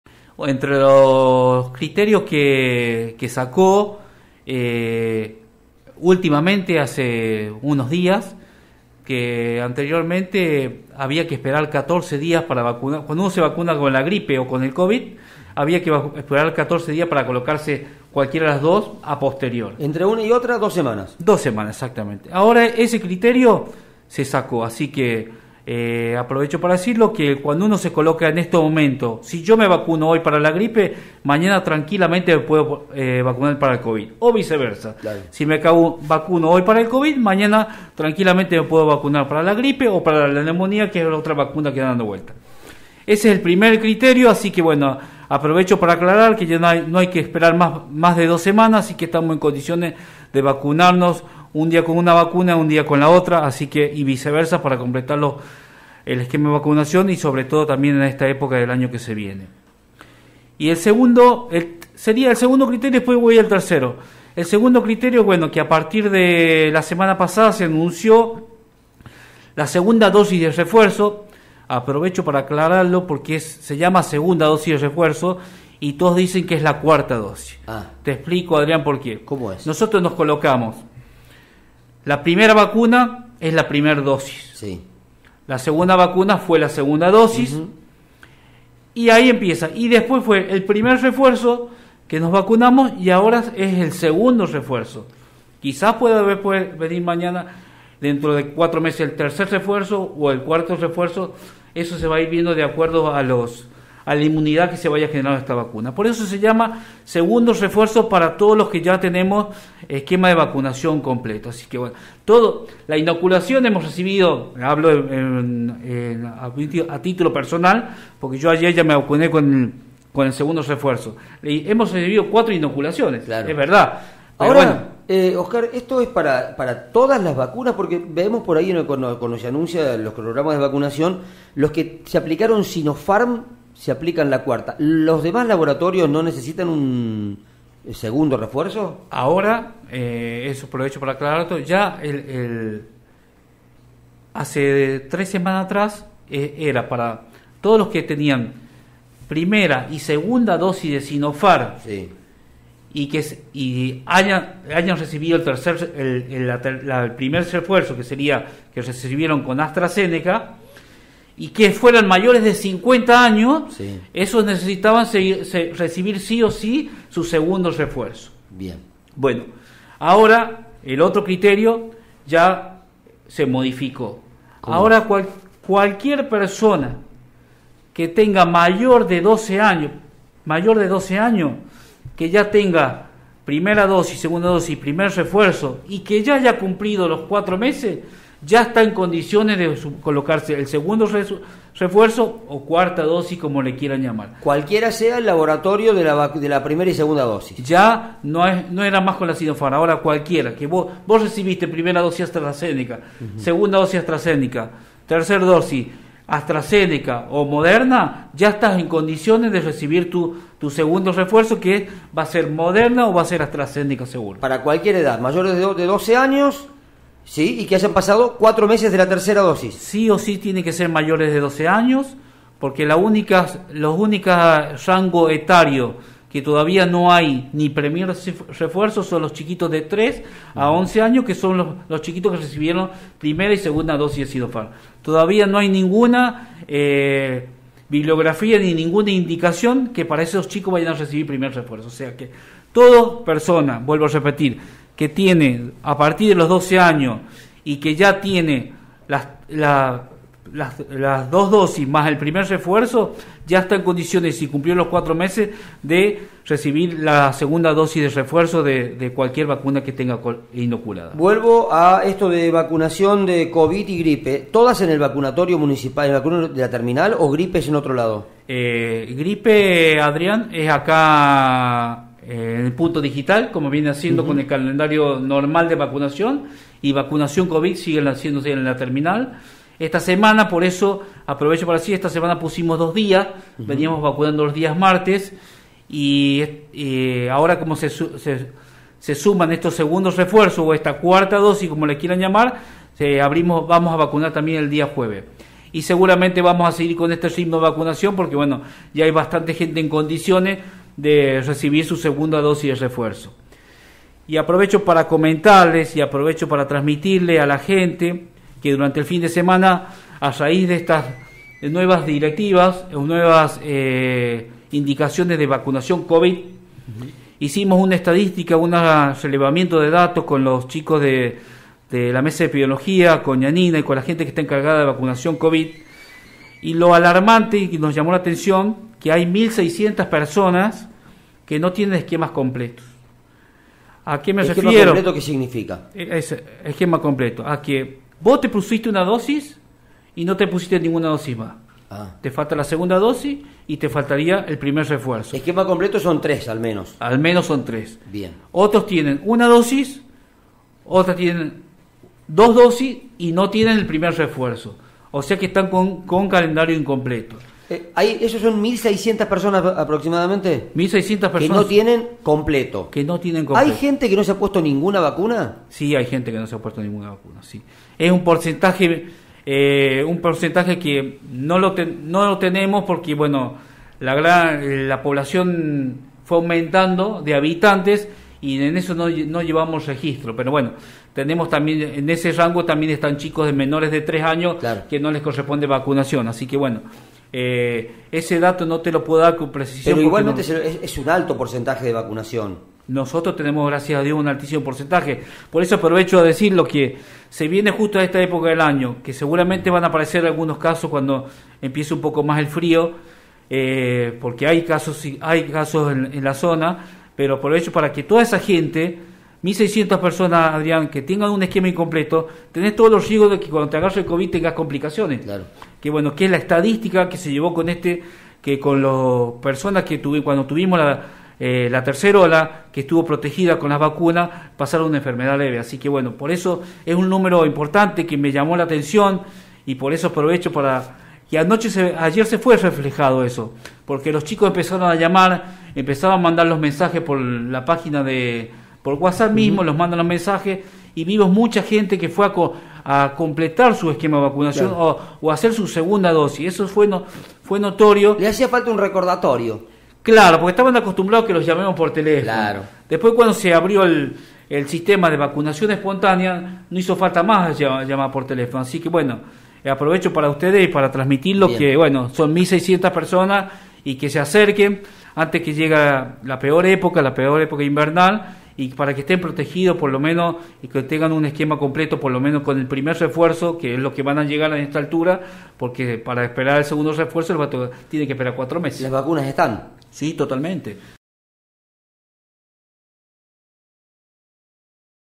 Oscar Gómez
El Director del hospital municipal San Roque, visitó los estudios de Radio Sudeste para aclarar los cambios de criterios para la aplicación del segundo refuerzo.